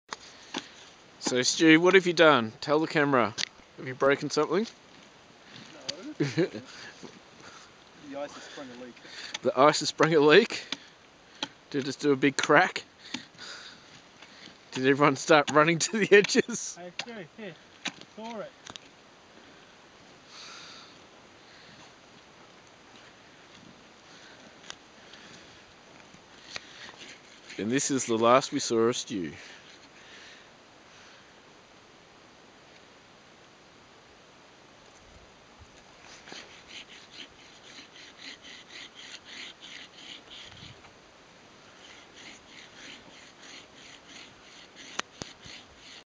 How thick is the ice? Part 1
Probably not the best way to go about it, but we needed to make a pilot hole to get the ice-saw in!